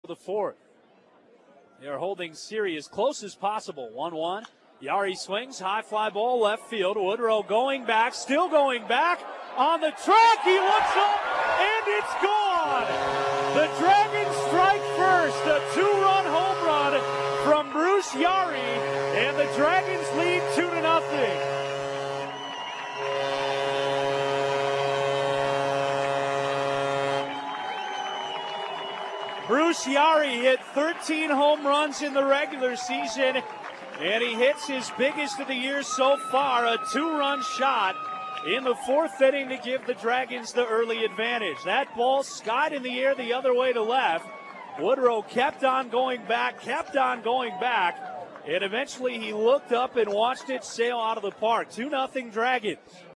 Dayton's Yari goes yard in fourth
9/6/17: Dayton's Bruce Yari goes yard in the fourth inning, a two-run shot to left to give the Dragons a 2-0 lead over West Michigan